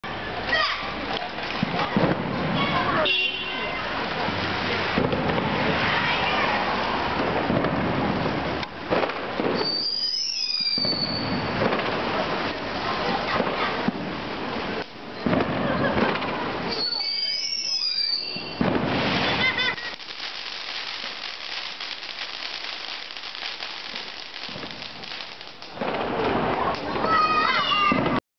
Fireworks on the road
More dangerous fireworks in Suzhou